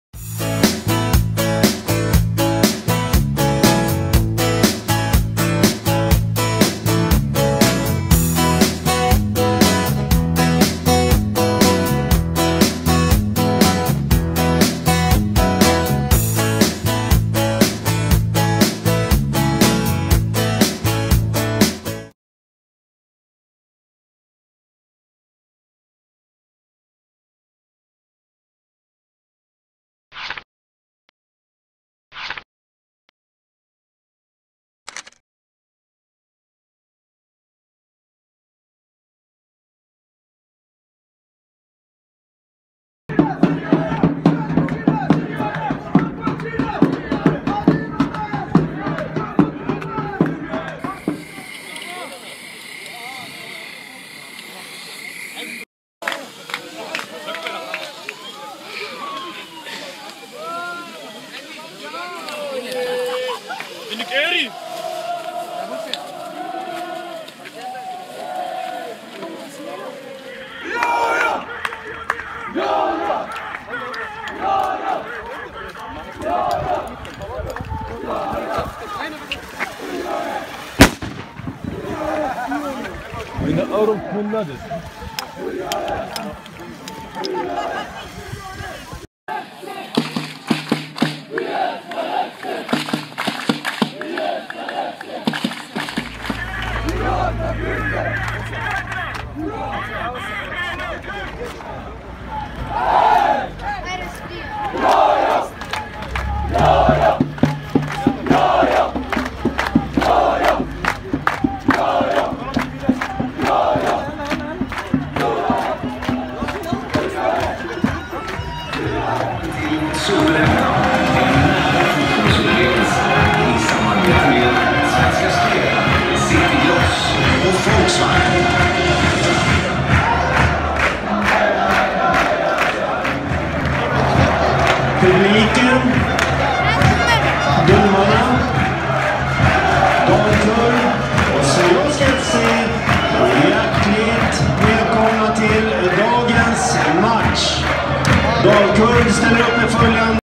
Syrianska vs Dalkurd 2016 (1-2)
Syrianska tog ledningen med 1-0 genom success i första halvlek.I andra halvlek gjorde Dalkurd mål genom Löfkvist (1-1). Syrianska fick chanser men satte inte de.Dalkurd fick 4 hörnor irad och tillslut klev inhopparen Leonard Pllana in och vände matchen i sista sekunderna för Dalkurd och matchen slutade 1-2 till Dalkurd.Det blev stökigt på läktarna efter matchen